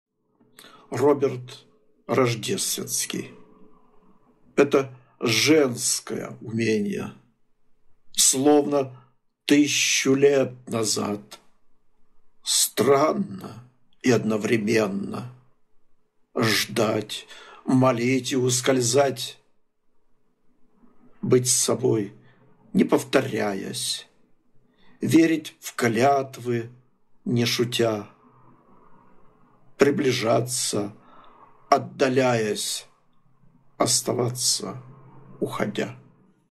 Это женское уменье. Роберт Рождественский#shorts
Стихотворение Роберта Рождественского "Это женское уменье" о загадочной женской душе. Смотрим, слушаем, комментируем...